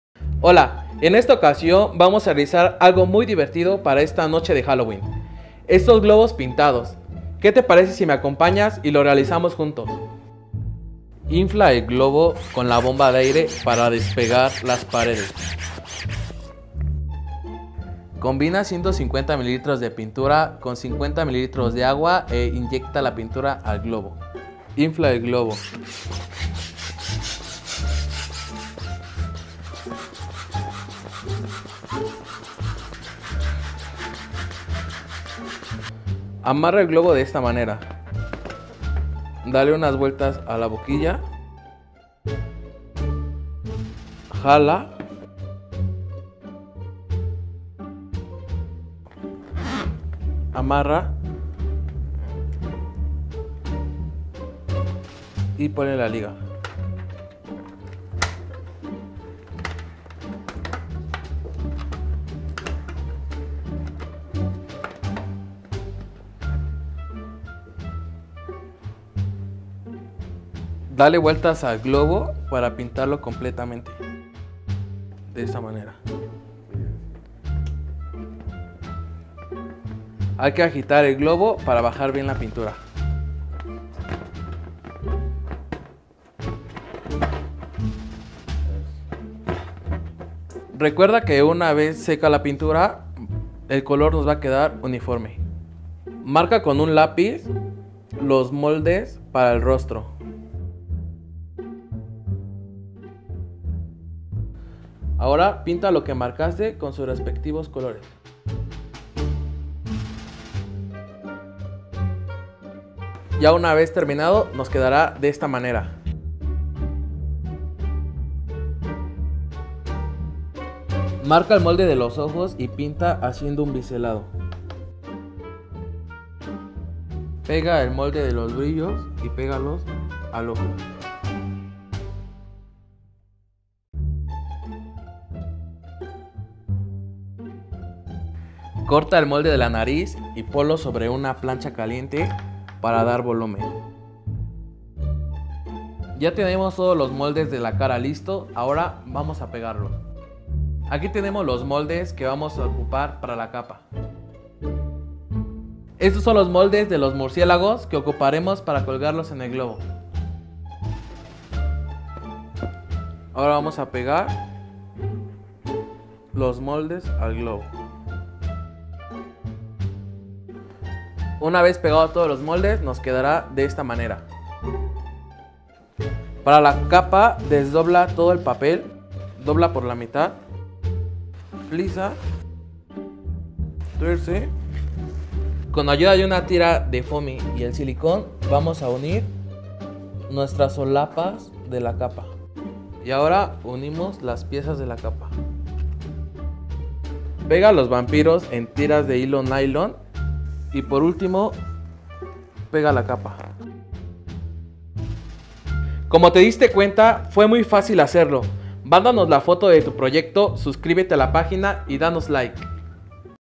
GLOBO VAMPIRO CON MURCIÉLAGOS
Aprendo cómo puedes hacer este Globo Vampiro con Murciélagos para este Haloween !!